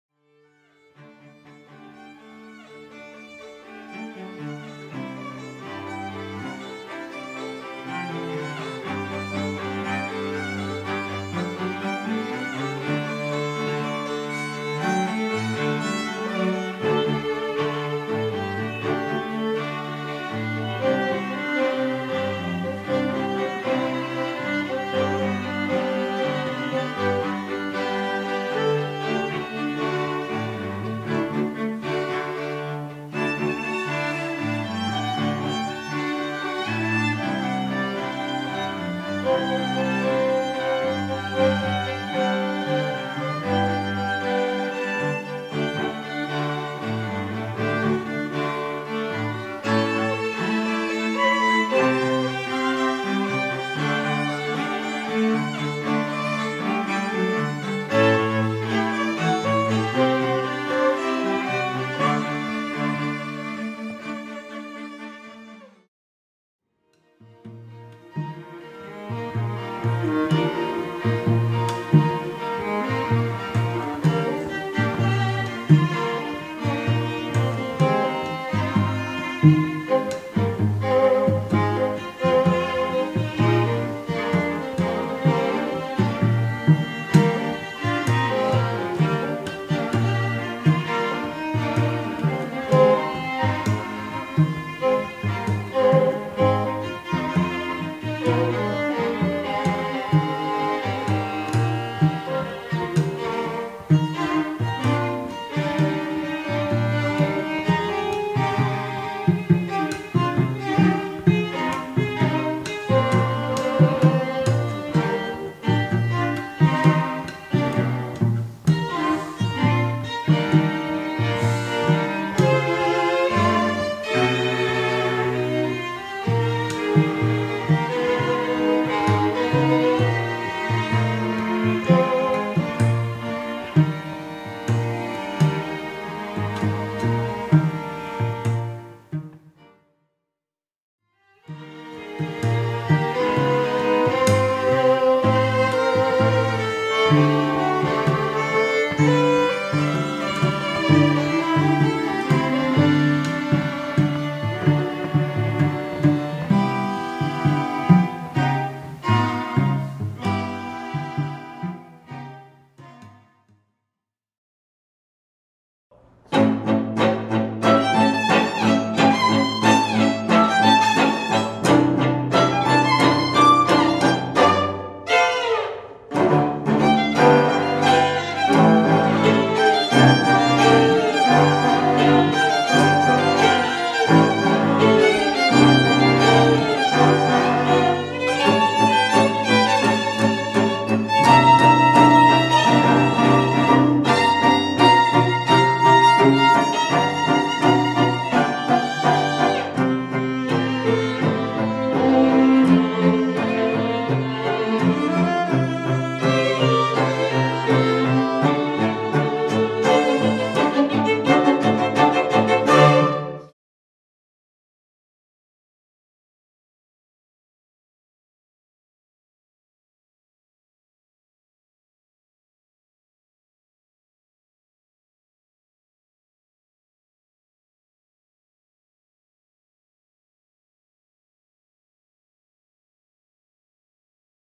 Yaletown String Quartet Aug 24.mp4
Come out to the OneMatch Cafe on Wednesday, August 24, 2011 to hear Vancouver's Yaletown String Quartet perform FREE!!

This video highlights 3 pieces performed by YSQ, as well as the importance of registering yourself as a stem cell donor thru Canadian Blood Services OneMatch program